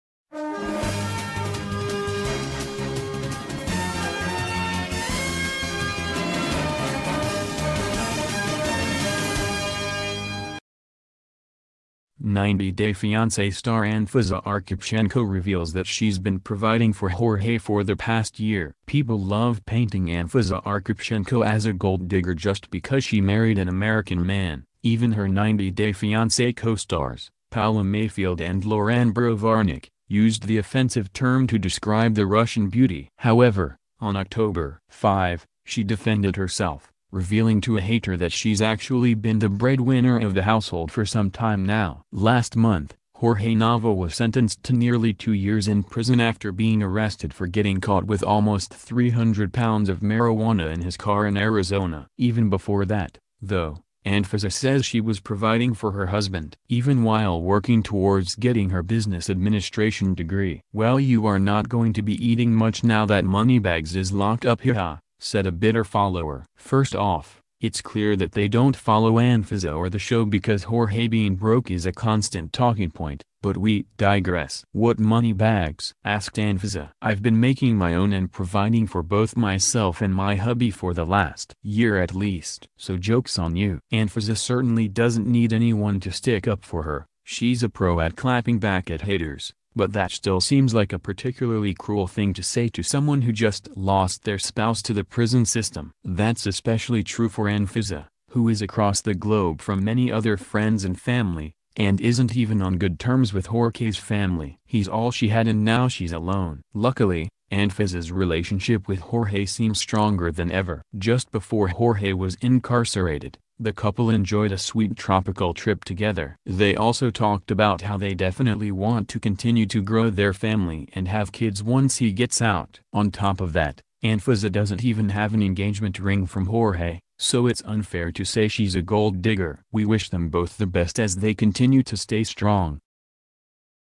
'90 Day Fiancé' Star Anfisa Arkhipchenko as a gold digger just because she married an American man
'90 Day Fiancé' Star Anfisa Arkhipchenko Reveals That She's Been 'Providing' For Jorge For The Past Year.